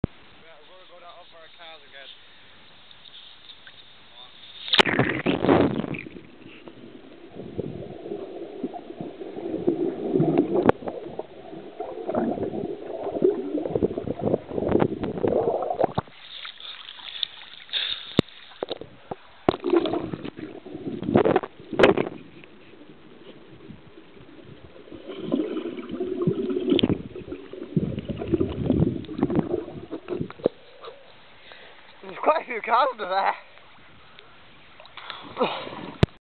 hodbarrow cars part 3
the first car is a ford fiesta the second one is a old metro there is 2 more I did not get on video the third is a vw golf and a rover and more